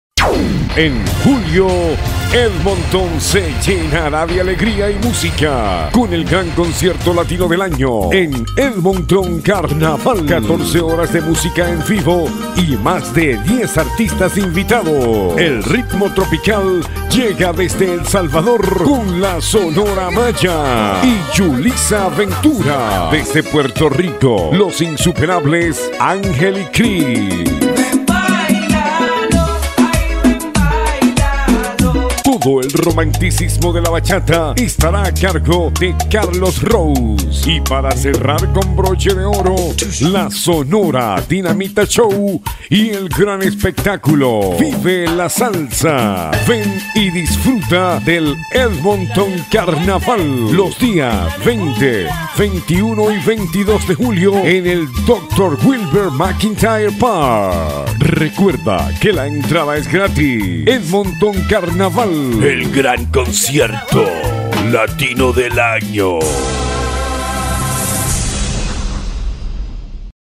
Edmonton Carnaval 2018
Ven y disfruta del mega concierto latino en el Edmonton Carnaval 2018
🌟La entrada es gratis 🌟
➤ 20, 21 y 22  de julio de 2018 ♥
➤ Dr. Wilbert Mcintyre Park 👈